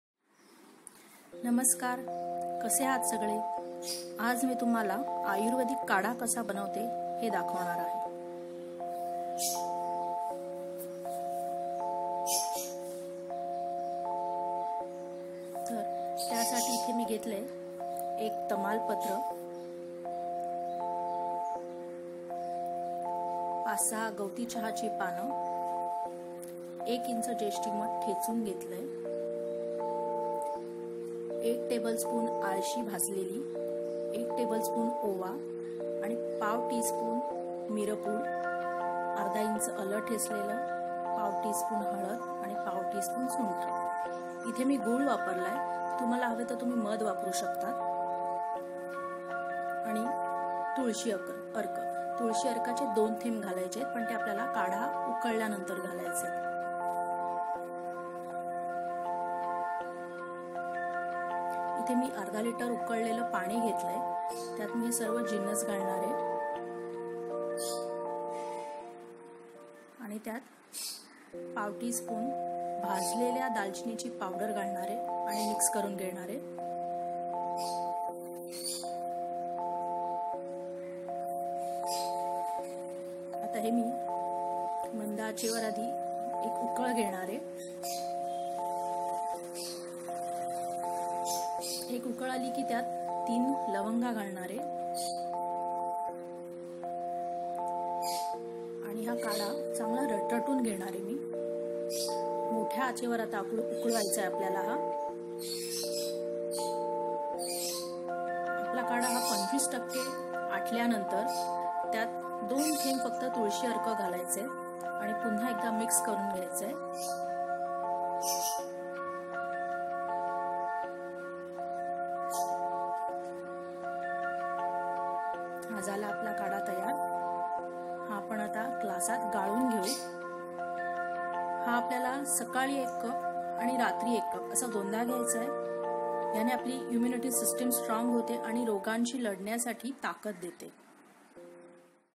Ayurvedic kadha| आयुर्वेदिक काढा | immunity booster kadha |घरगुती काढा
500मिली उकळलेले पाणी +1तमालपत्र+ 5-6 गवती चहाची पात+1इंच जेष्ठीमध ठेवलेली +1चमचा भाजलेली आळशी+1चमाचाओवा + 1/4 tspn काळीमिरी पावडर + 1/2इंच आले ठेचलेले + 1/2 tsp हळद +1/2tsp सुंठ पावडर + आणि गूळ कीव मध + 1/4 tspn भाजलेली दालचिनी ची पावडर मिक्स करून गॅस वर उकळत ठेवावे.
                एक उकळ आली की 3लवंगा घालाव्या. काढा मोठ्या आचेवर उकळून घ्या. 25% उकळल्यावर त्यात 2थेंब तुळशी अर्क घालून चमच्याने ढवळून घ्या. झाला काढा तयार.
                 खोकला, ताप किंवा कफ असल्यास  सकाळी एक कप व रात्री एक कप शक्य तितक्या जास्त गरम गरम प्यावा.
           काळजी घ्या आणि सुरक्षित रहा🙏.